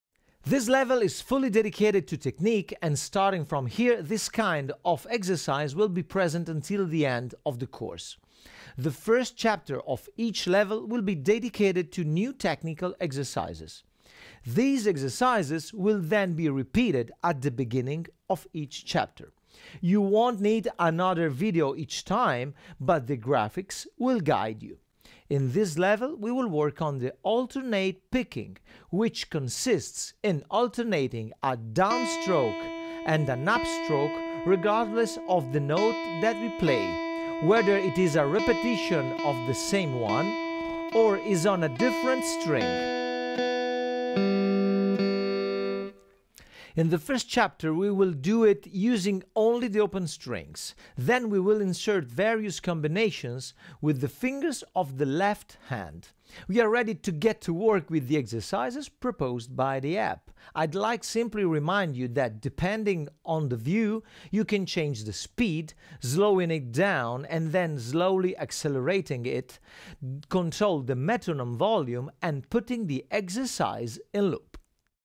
Level 9 Visual Note Beginner Course Introduction by Massimo Varini: Alternate Picking.
Welcome to the Visual Note Guitar Beginner Course. In this clip, our excellent teacher @guitartribe introduces Level 9: " Alternate picking".

Download the Visual Note app and start playing the guitar!